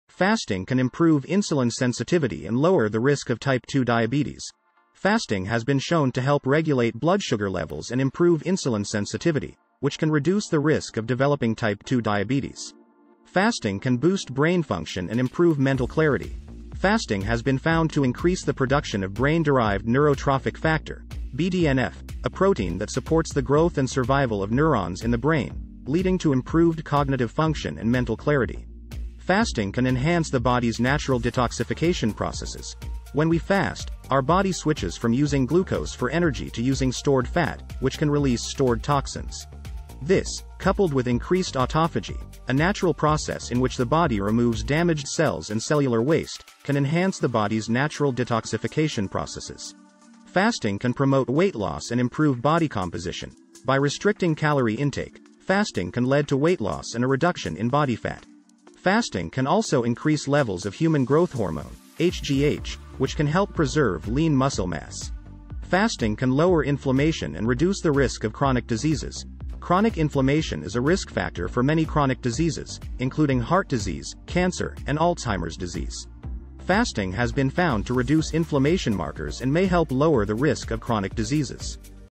5 Surprising Benefits of Fasting You Need to Know
Fasting is an ancient practice that has gained popularity in recent years, thanks to its many health benefits. In this video, we'll explore five surprising benefits of fasting that you may not know about, including improved insulin sensitivity, enhanced brain function, natural detoxification, weight loss, and reduced inflammation. Whether you're new to fasting or a seasoned pro, you won't want to miss these fascinating facts.